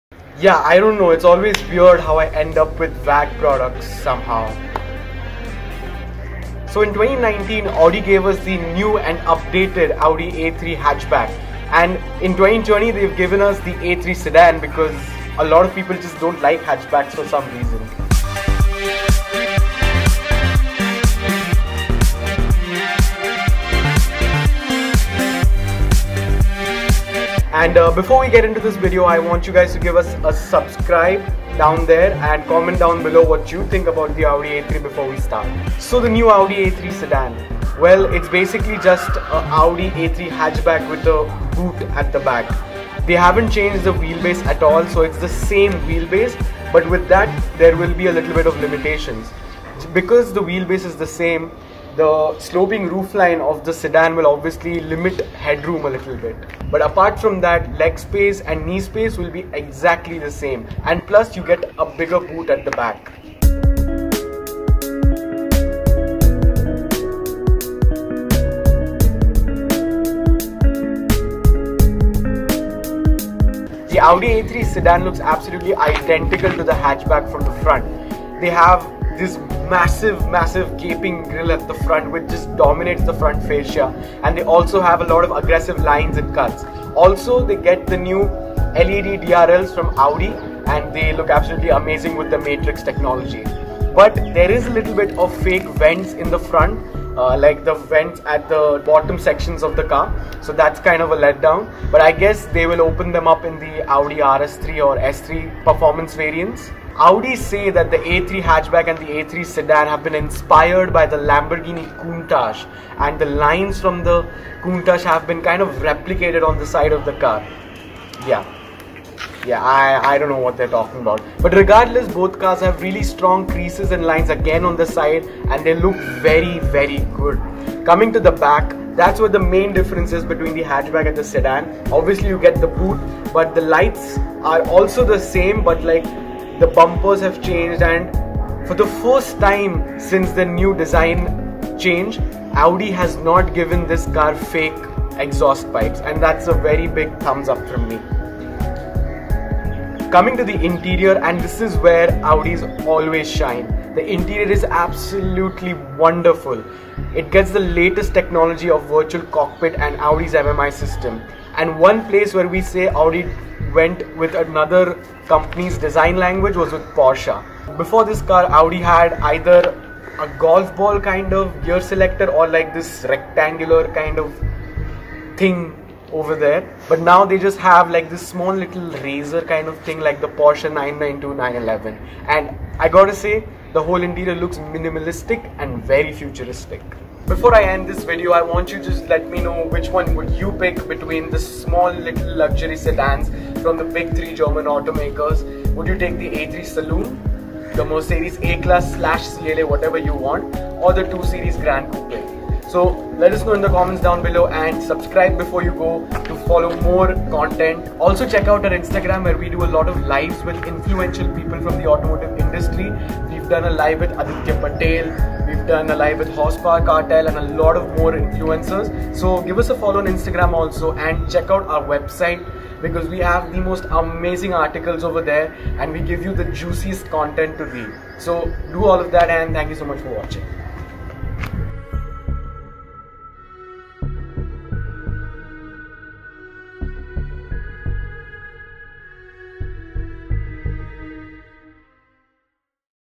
2020 Audi A3 Sedan Overview | Inspired by a Lamborghini Countach? | UpShift
The Audi A3 Sedan has always been a mediocre car with decent looks and okayish luxury. But for 2020 they have really upped the ante and given it a complete overhaul with the latest design philosophy from Audi and also the latest version of the Audi's Virtual Cockpit and MMI system. The new Audi A3 Sedan is all geared up to go against the latest competition from BMW and Mercedes. It competes with the Mercedes A-Class Sedan and Mercedes CLA Coupe and also the BMW 2 Series Gran Coupe.